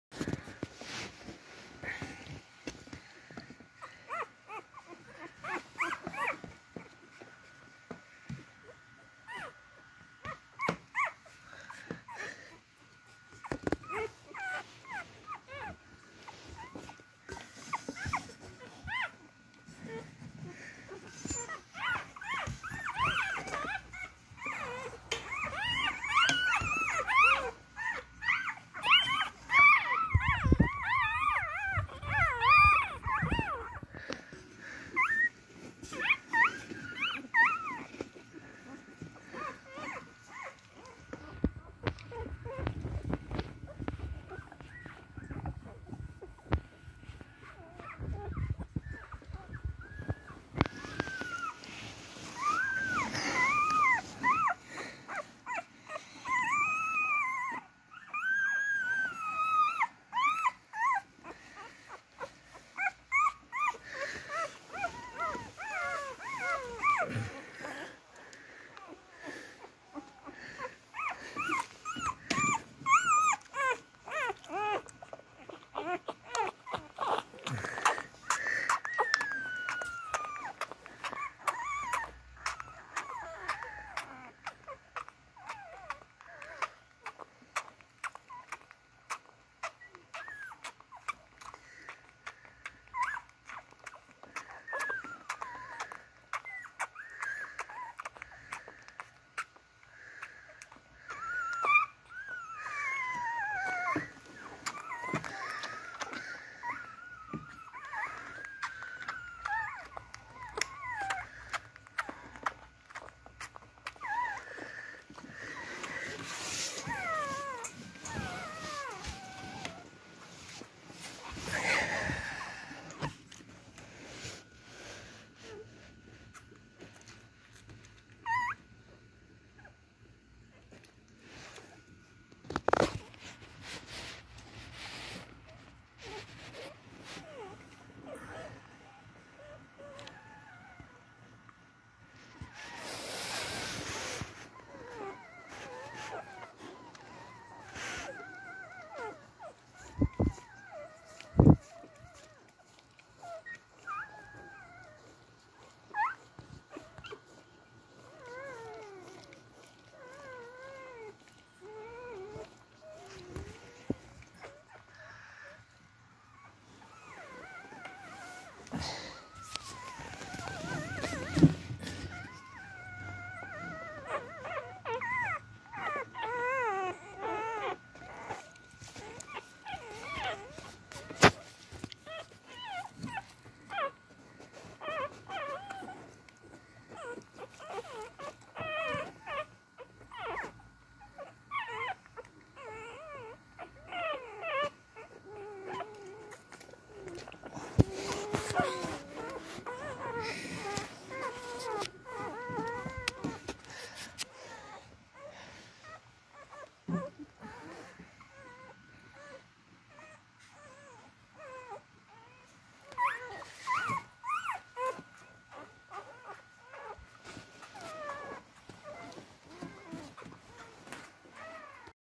Sweet cute beagle puppies crawling around for their meal to get to their mother
These sweet adorable beagle puppies are looking for their mom to eat. Some have to put in a lot of effort to find their way to the milk.